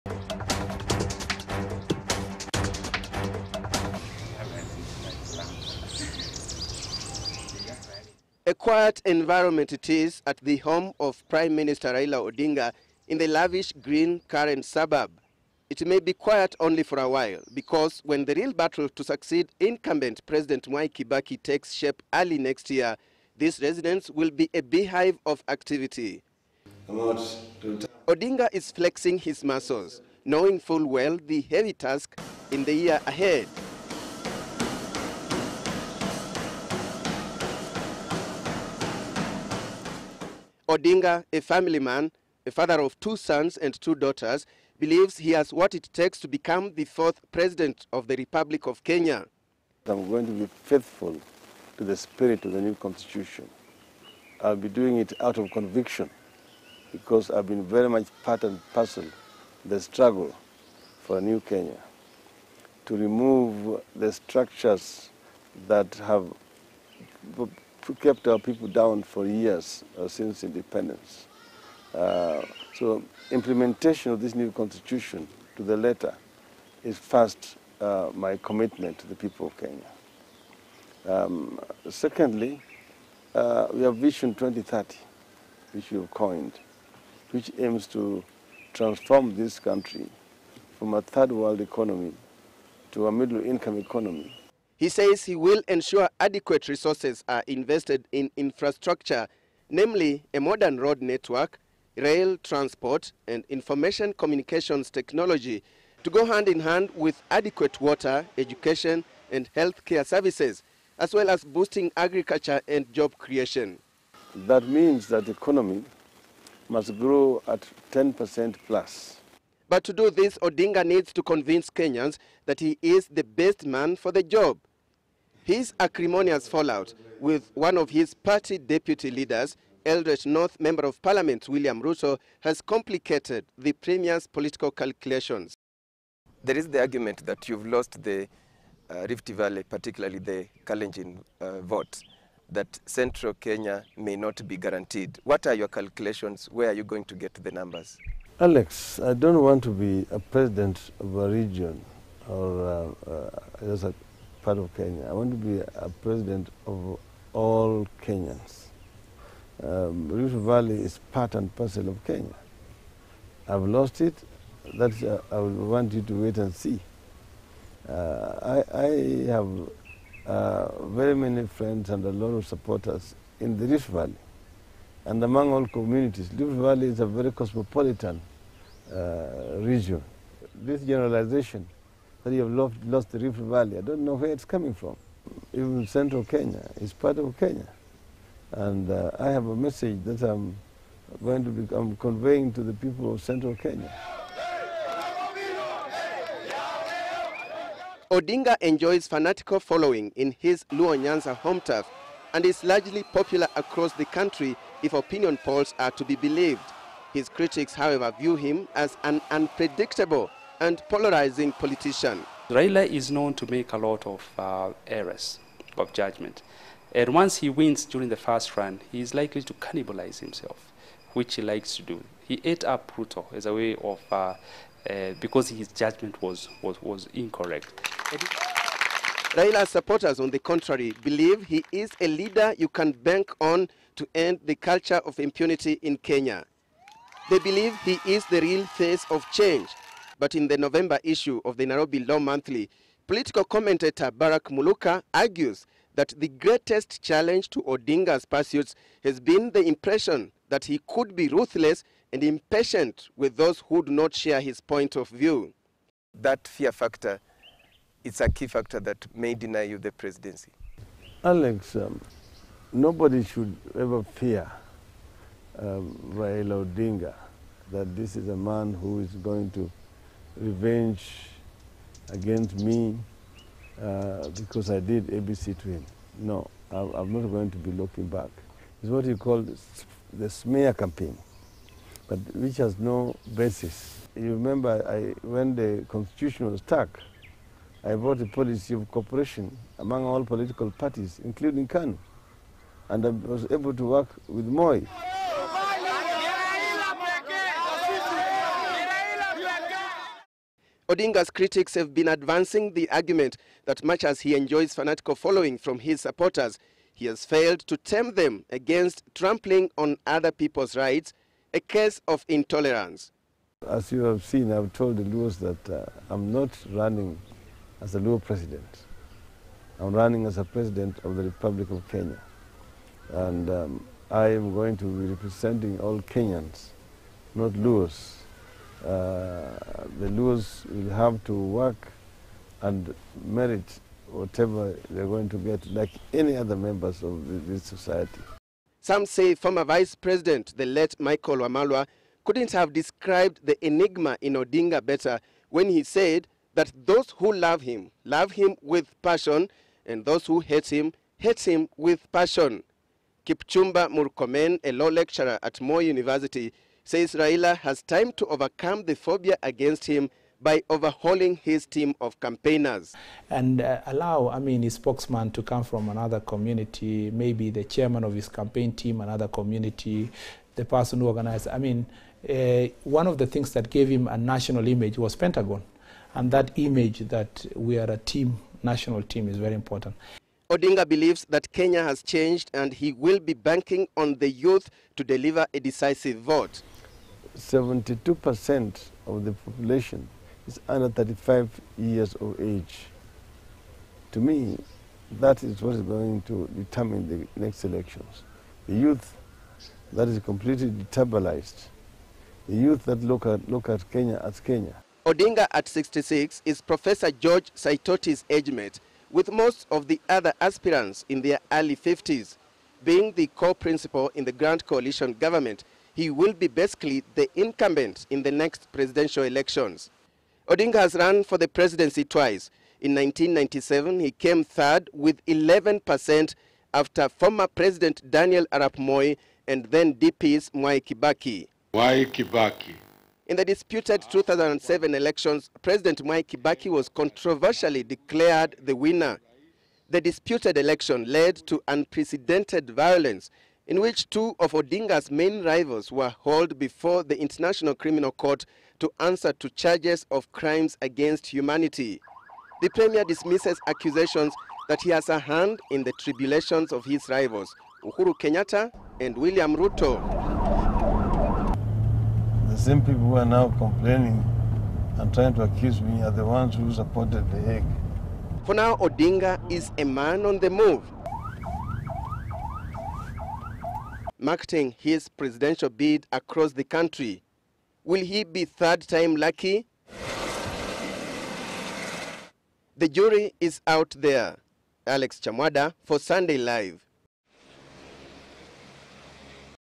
Kibaki Succession: Raila Odinga
THE KIBAKI SUCCESSION: Focus on PM Raila Odinga
On our Kibaki succession series, we put Prime Minister Raila Odinga who has in the recent past been very much in the spotlight firmly under the microscope tonight. Will it be 2012 or never for the Prime Minister as far as his chances of ascending to power go? Well, analysts and even recent opinion polls say he is the man to beat. Odinga is a man many love to hate. His critics are as eloquent in their dislike as his supporters are in their praise. Our associate editor Alex Chamwada spent time with the Prime Minister at his Karen residence and where Odinga spoke of his chances.